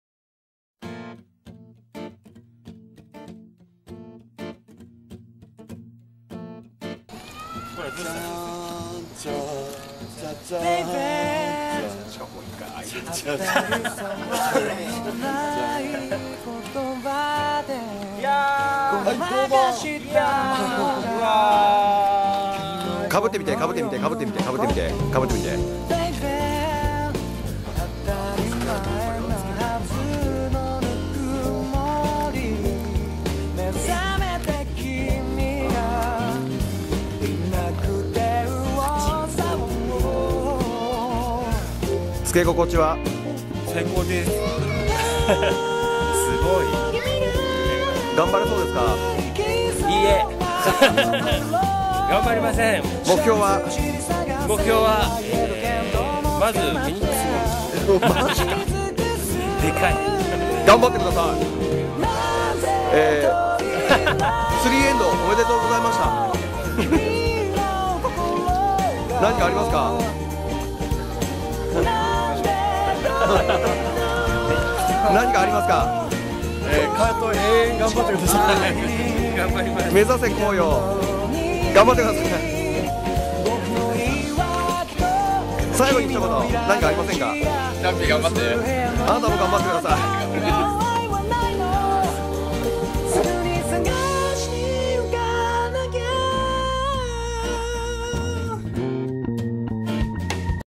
オレンジヘルメットの思い出
カヤックの師匠　「シャーク」「ピース」「ホッパー」より
カートという技ができたときに、新品のオレンジヘルメットをいただいたときの動画です。
（ショウも写っています）
ずっと大事にしていきます！
カヤックに人生のすべてをかけて、楽しむことをここに誓います♪